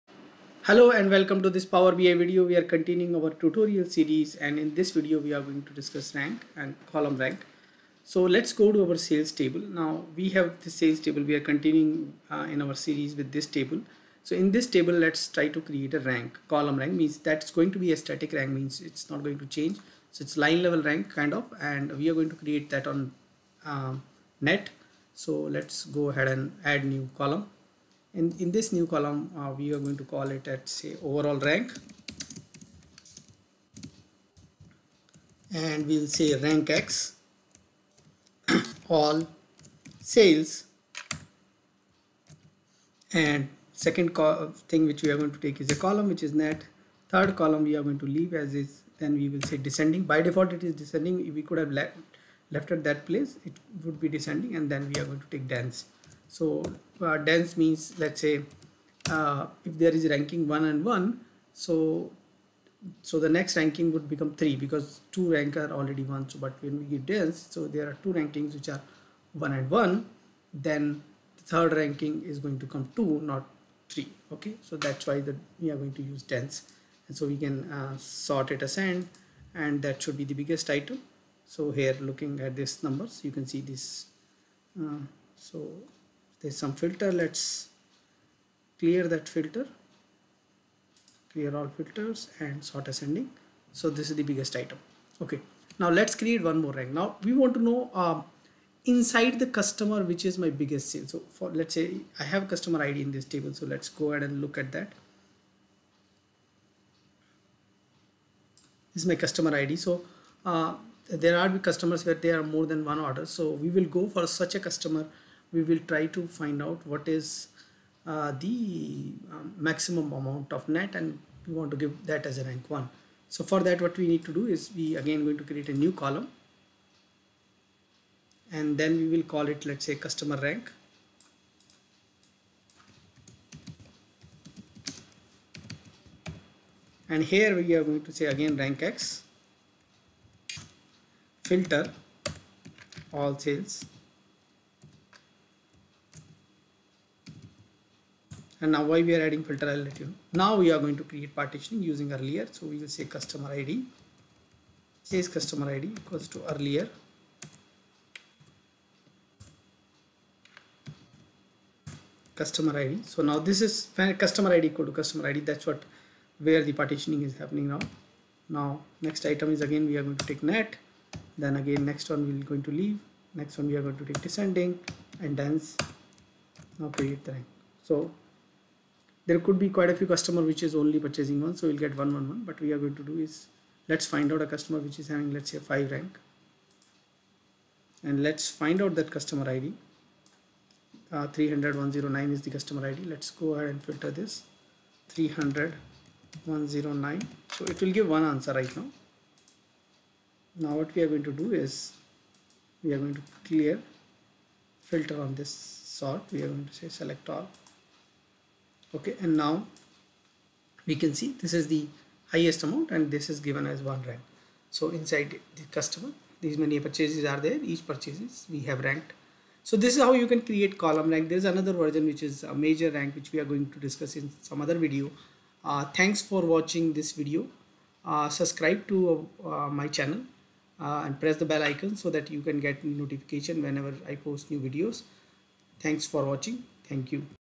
Power BI Tutorial Series for Beginners Part 39: Column Rank (RANKX)
Power BI Rank, DAX Rank, Column Rank, Rank, Power BI Rankx, DAX Rankx, Rankx
Power BI Tutorial Series for Beginners Part 39: Column Rank

00:00 Introduction Power BI Rank
00:30 Power BI Column Rank
03:00 More Examples Rankx